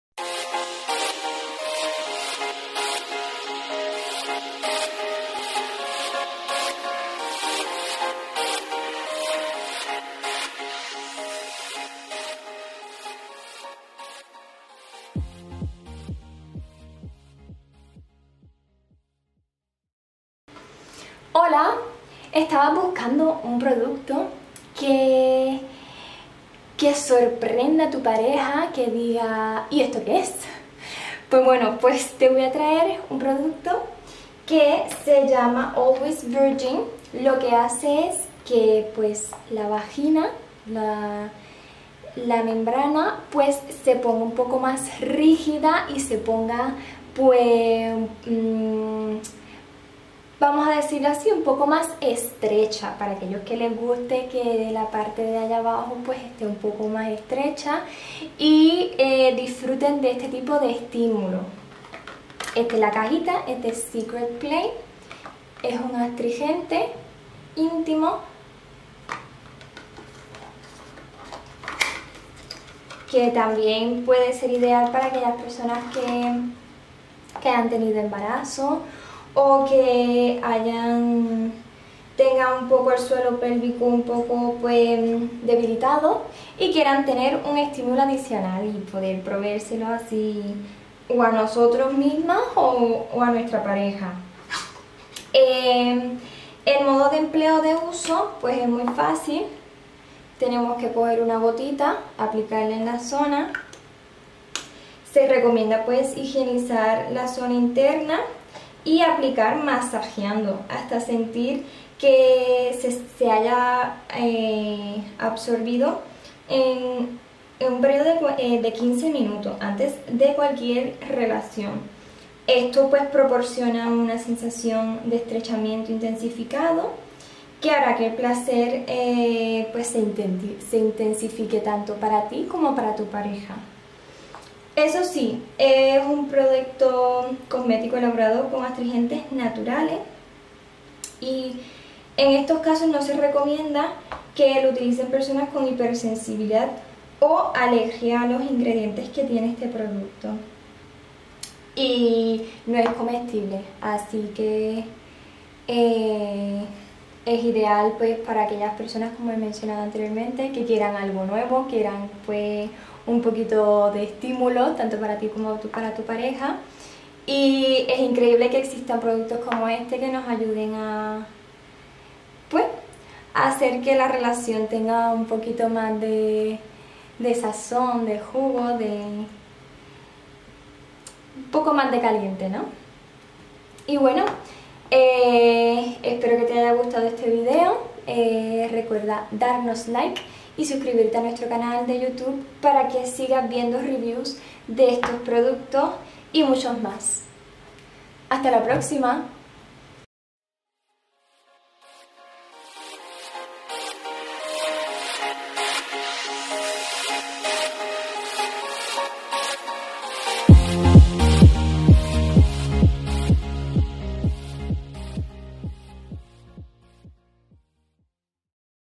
Como ESTRECHAR la VAGINA? ⚡️ || Always virgin intimate astrigent
¿Te gustaría experimentar de nuevo la sensación que tuviste en tu primera vez? ALWAYS VIRGIN INTIMATE ASTRINGENT es un astringente íntimo compuesto de ingredientes naturales que proporciona una sensación de estrechamiento de los músculos vaginales.

ALWAYS VIRGIN INTIMATE ASTRINGENT proporcionan una agradable sensación de estrechamiento sin eliminar la lubricación vaginal durante las relaciones sexuales. El gel de estrechamiento vaginal es una crema especialmente concebida para ayudar a tonificar los músculos flácidos y a estrechar el conducto vaginal, gracias a su efecto astringente. Con ingredientes naturales podras conseguir sentir la vagina mas firme y fuerte.

Con este producto descubrirás sensaciones nuevas que te llevarán al paraiso del orgasmo. ¡Un efecto revolucionario para sensaciones increíbles, utiliza el gel de estrechamiento vaginal ALWAYS VIRGIN INTIMATE ASTRINGENT y mejora tu vida sexual!

No dudes en probar ALWAYS VIRGIN INTIMATE ASTRINGENT.  Puedes encontrarlo al mejor precio en nuestro SexShop online, en el siguiente enlace: